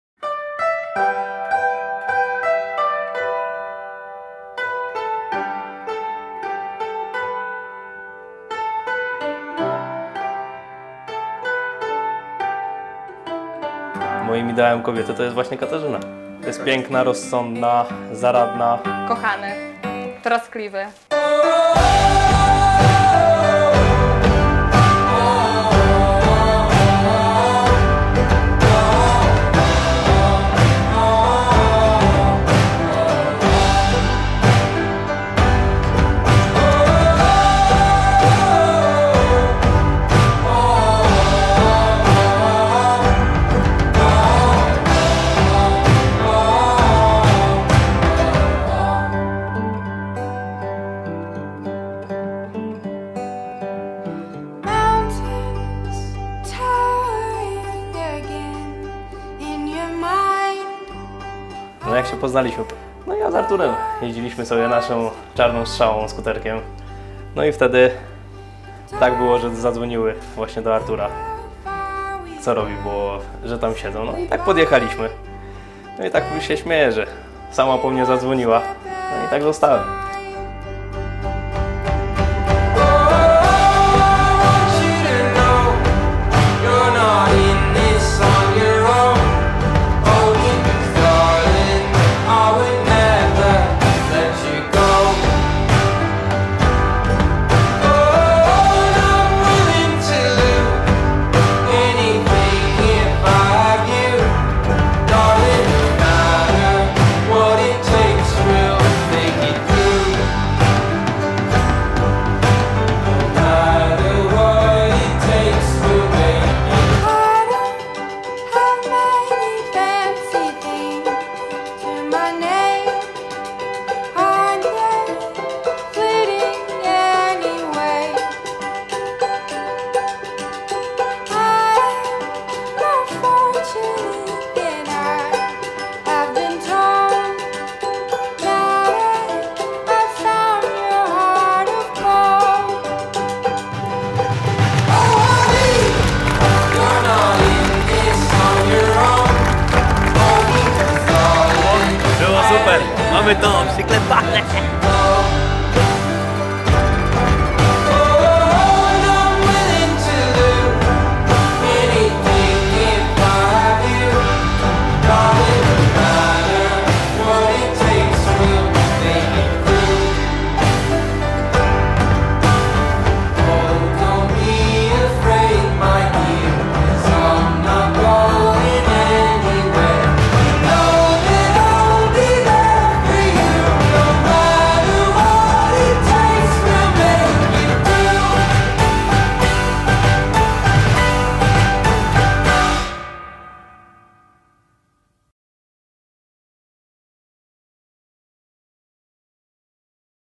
Kasia i Mateusz | Teledysk ślubny | BŁAŻEJÓWKA | Wesele w stylu rustykalnym | [4K]
realizacja STUDIO PIEŃKOWSKI
Ślub Kasi i Mateusza odbył się w kościele parafialnym w Górznie koło Garwolina.
Para młoda gościła swoich gości w pięknej rustykalnej altanie "Błażejówka".
Grał i śpiewał zespół LuXuS